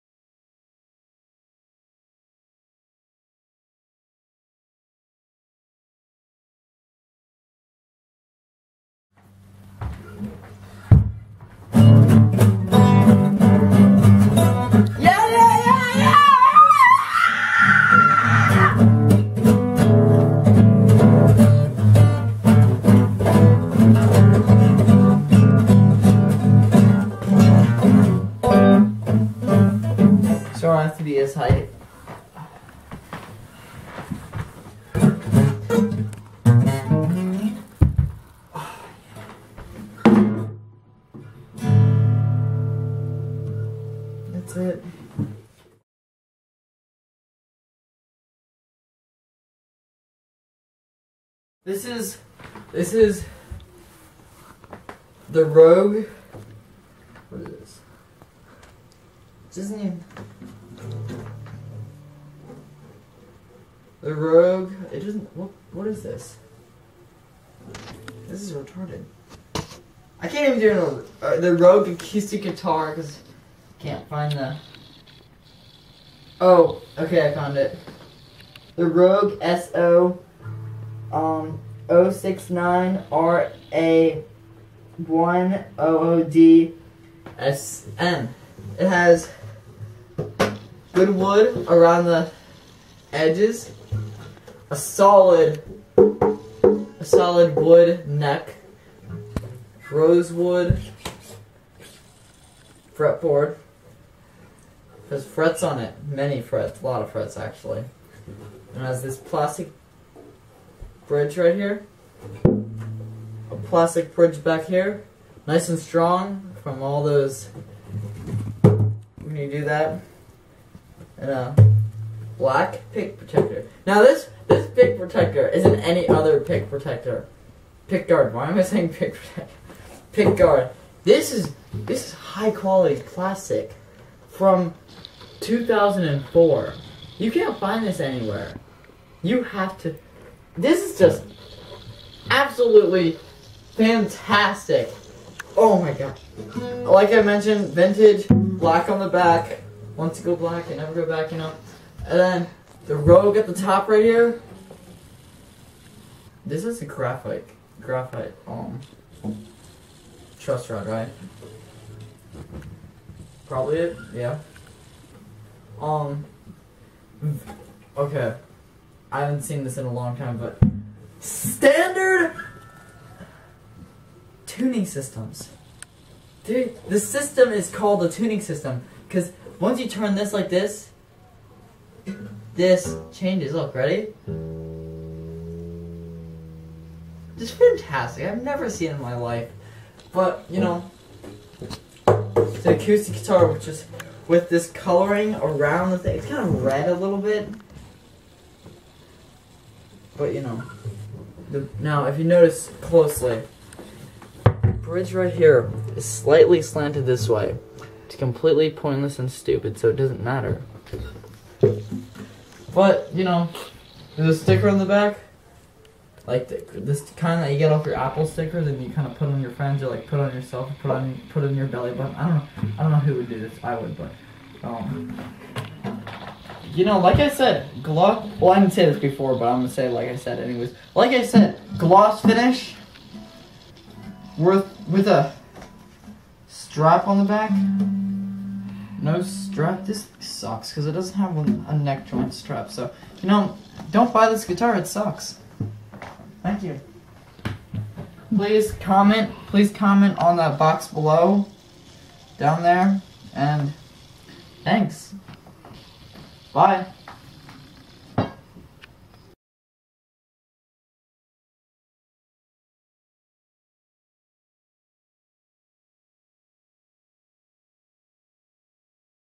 Reviews with Noah Young - The Rogue Acoustic Guitar
Noah decides he is going to take on reviewing the Rogue acoustic guitar, telling you the ups and downs of the guitar and wether or not you should buy it!